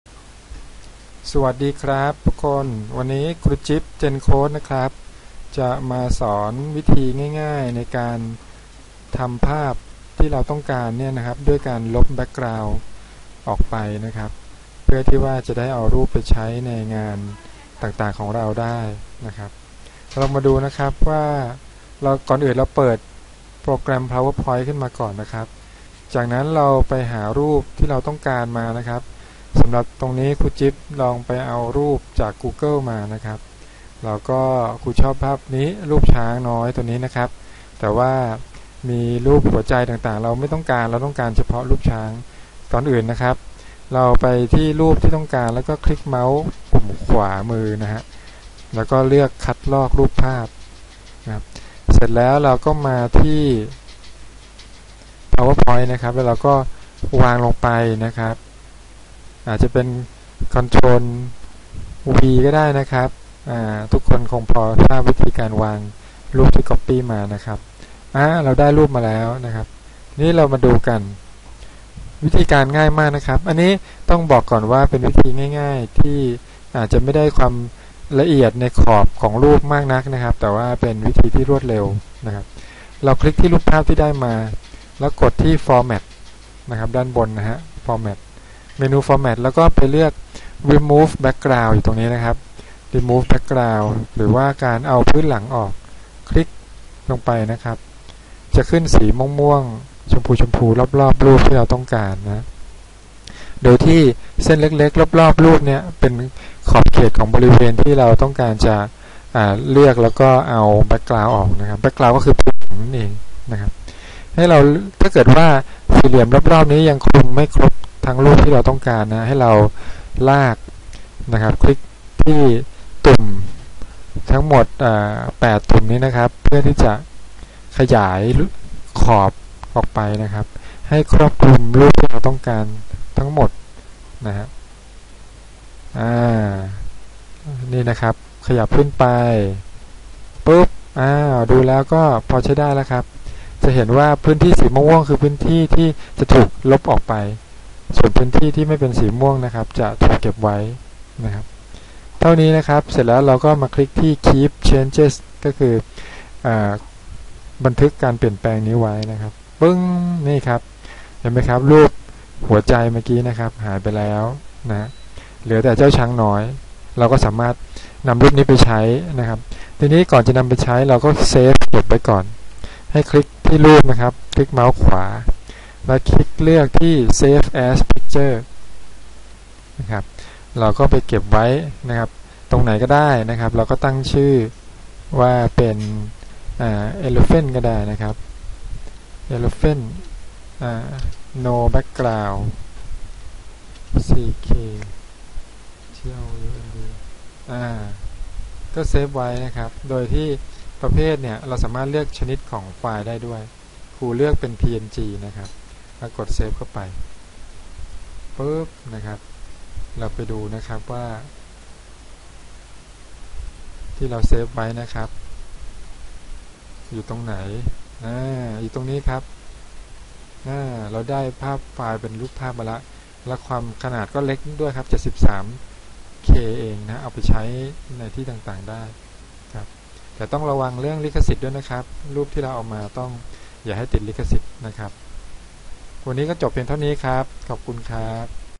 การลบพื้นหลังของภาพ ด้วย PowerPoint ง่ายๆ (remove background)
มาชมวิธีลบพื้นหลังของภาพที่เราต้องการ โดยใช้ PowerPoint กันครับ เด็กๆ ก็ทำได้ครับ ง่ายๆ สไตล์ Gencode สอบถามเพ่ิมเติมได้ Line ID : gencode1